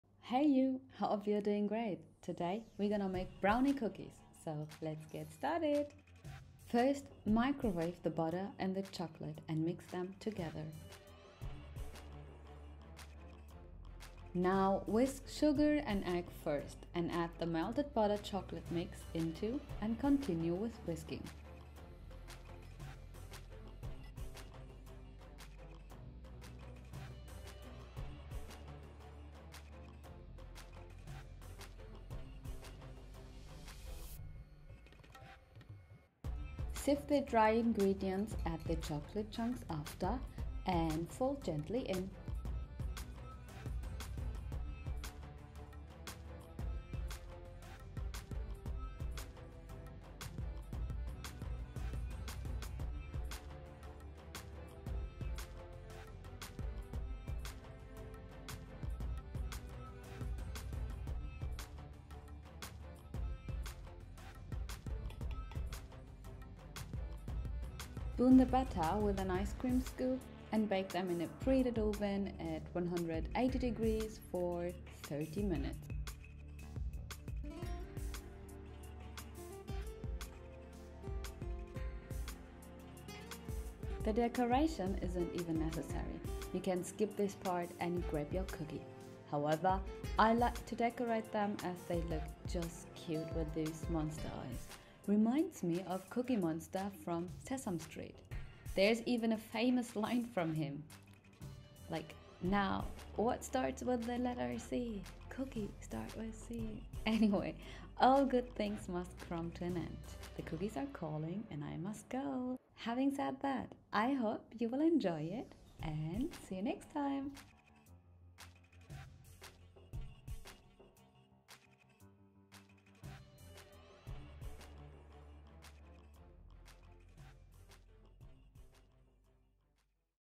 Brownie Cookies | WeeBee Cakery
Hey You! 🙃

Brownie meets Cookie! Super duper quickly done!

▷Ingredients (7 cookies) :

34g melted butter
110g melted dark chocolate
67g brown sugar
1 egg
50g flour
5g cocoa
2g baking powder
salt
35g chocolate chunks

180°, 13 min

Would be nice to get some feedback. 🙂